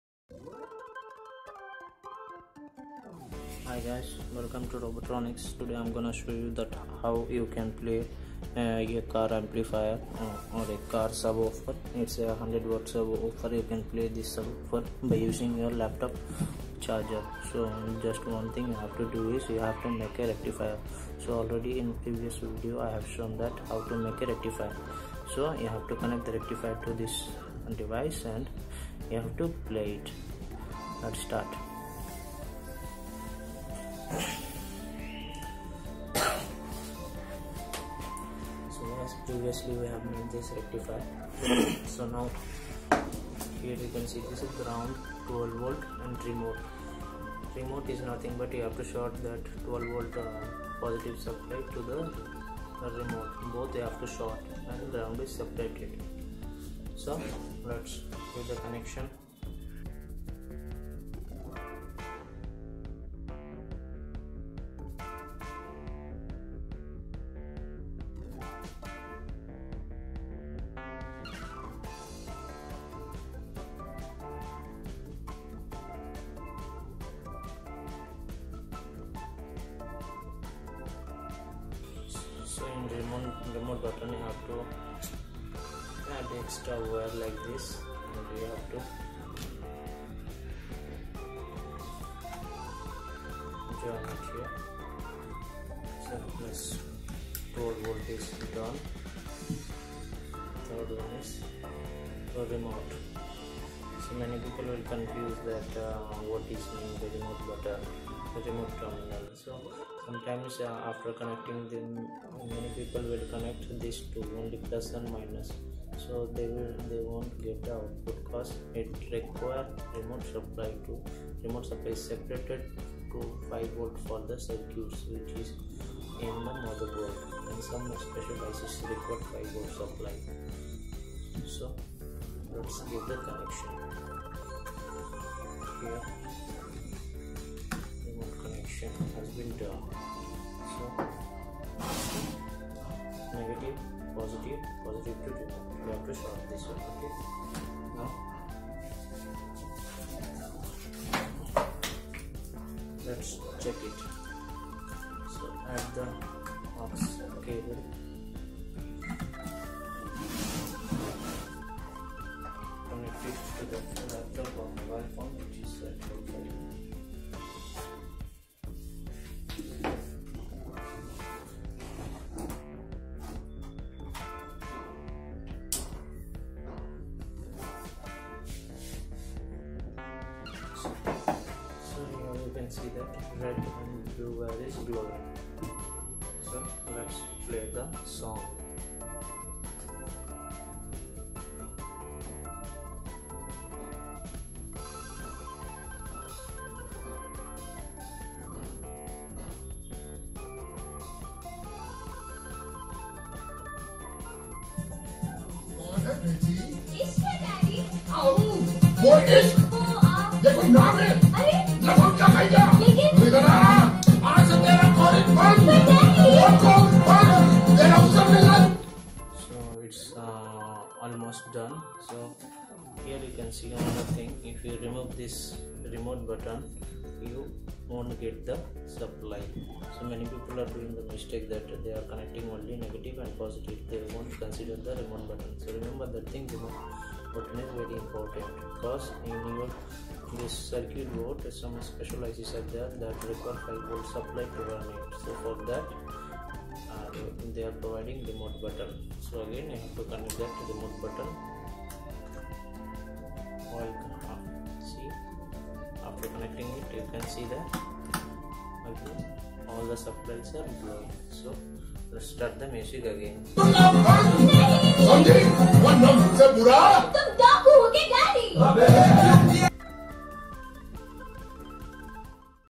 How to power a car amplifire using a laptop charger !
You can use your laptop charger a supply unit to run your car amplifier at your home,the thing is you have to make a rectifier and then the connection so in this video I have shown you the steps to make the connection using a laptop charger.